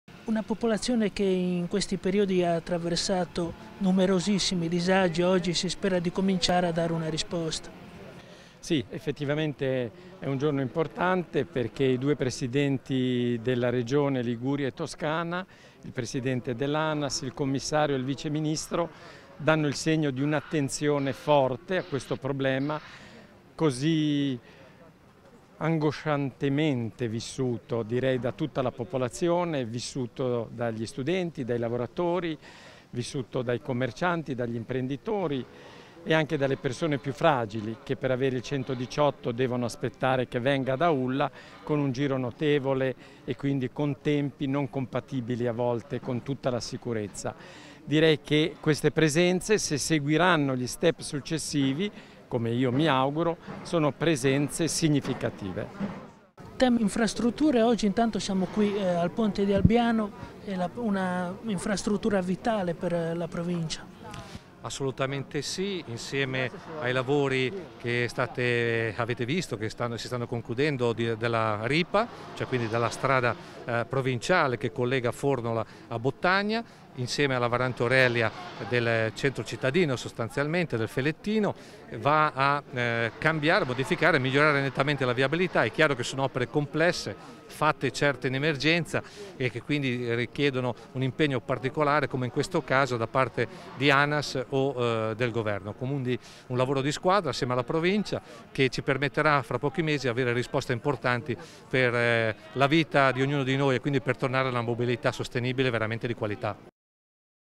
Ponte di Albiano e viabilità 04-05-2021
Soddisfazione da parte del Sindaco di Aulla Roberto Vallettini e del Presidente della Provincia della Spezia Pierluigi Peracchini dopo la visita di ieri al Ponte di Albiano del Vice Ministro alle Infrastrutture Alessandro Morelli. Il ponte dovrebbe terminare l’anno prossimo.